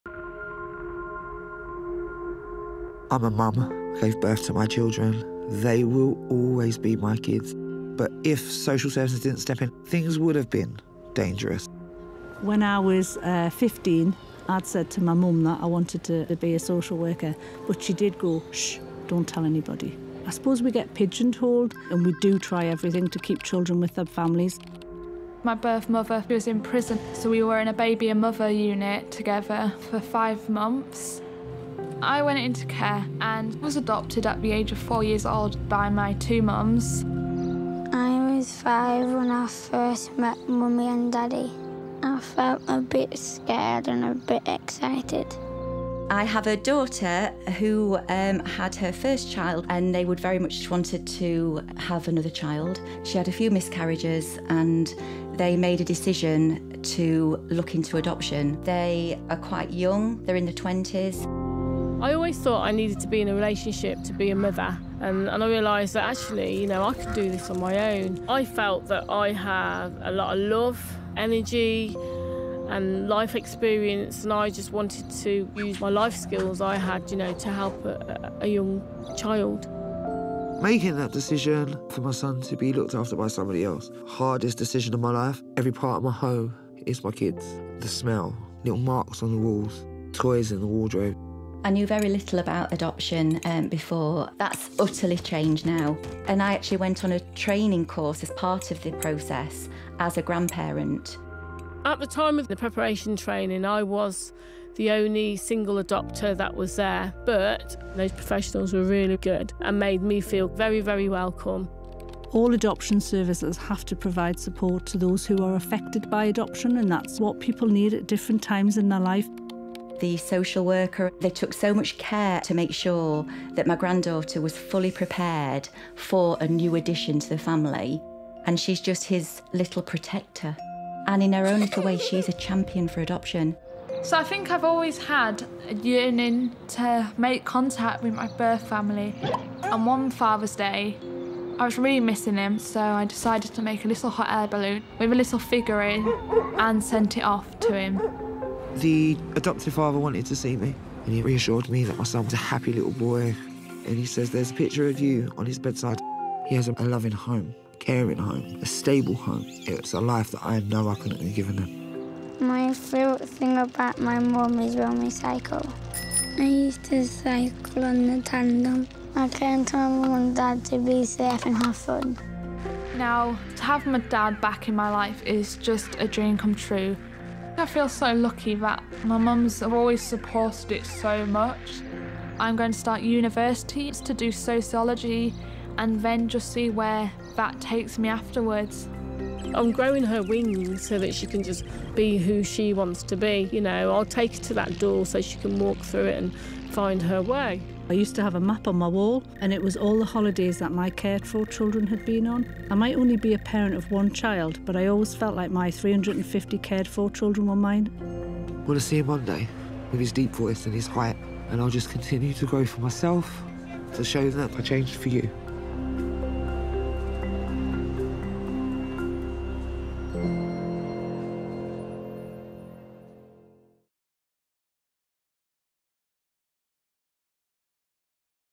National Adoption Week 2021 - Campaign Film
Illustrator and adoptive parent Garry Parsons has created a video animation for this year's National Adoption Week featuring the life stories and real voices of six people that have had their life changed by adoption.